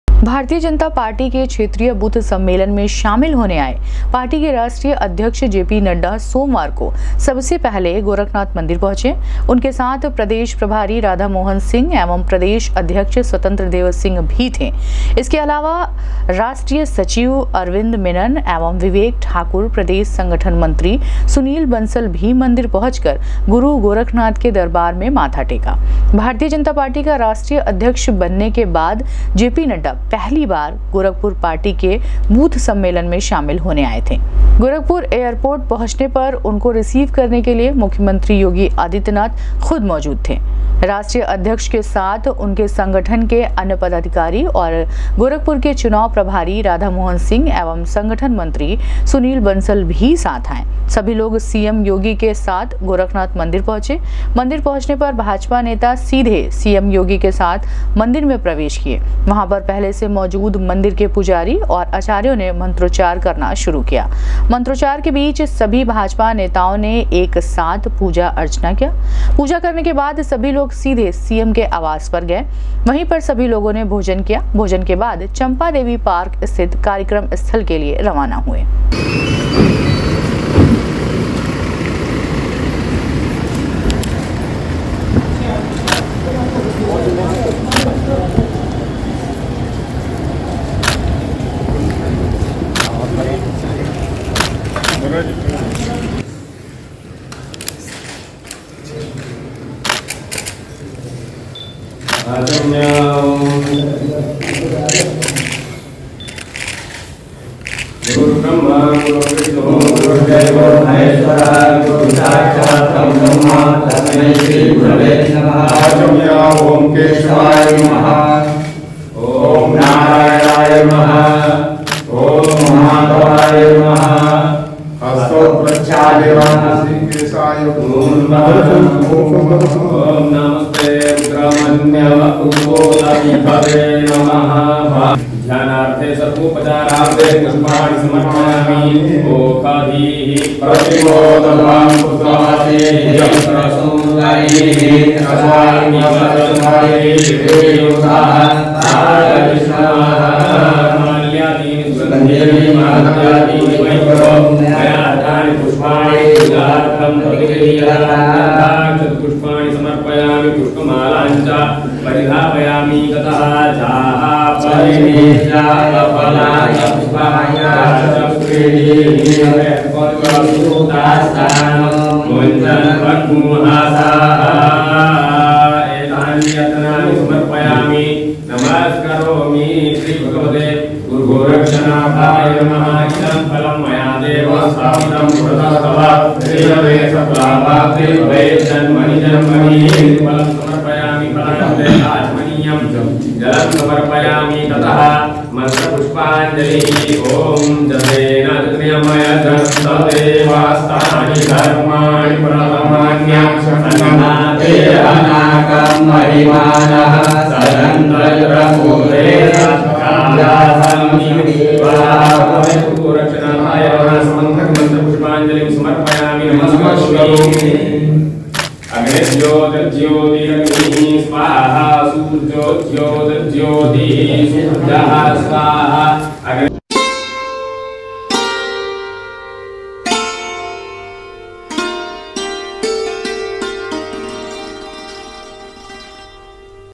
गोरखपुर पहुॅचे भाजपा के राष्टीय अध्यक्ष जे0पी0नड्डा ने गुरु गोरक्षनाथ की,की पूजा अर्चना,
गोरखपुर पहुॅचे भाजपा के राष्टीय अध्यक्ष जे0पी0नड्डा ने गुरु गोरक्षनाथ की,की पूजा अर्चना, ब्रम्हलीन महन्थ अवेद्य नाथ के प्रतिमा पर टेका मत्था।।